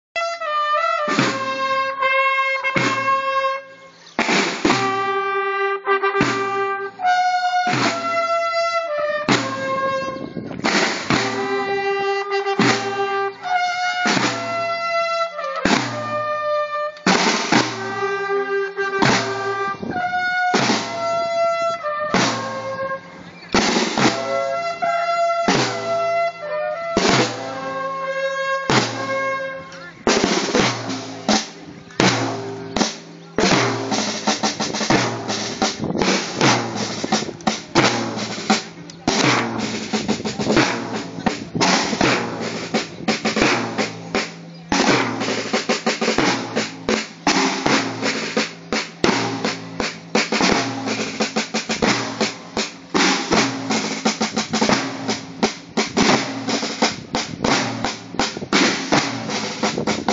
2012 - Últimos ensayos de la Banda
2 de abril de 2012. Últimos ensayos de la Banda de Cornetas y Tambores de Torrequebradilla para la procesión del Jueves Santo.